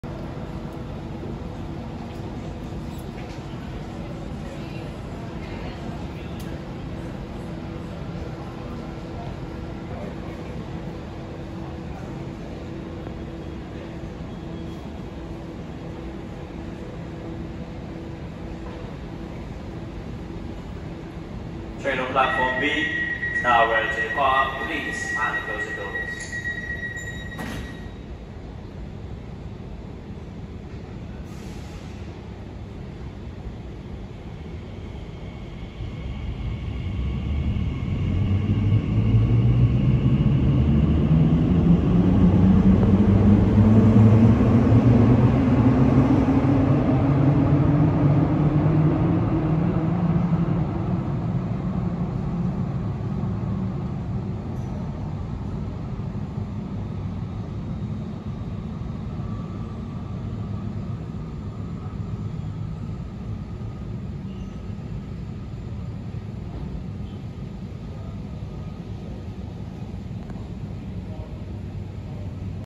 🚇 ELIZABETH Line | Train departing Canary Wharf Station.
New Elizabeth Line Class 345 Train departing Canary Wharf

📆Date filmed: May 2022
📍📍Location: Canary Wharf Station

Do not re-up, distributed any of my content.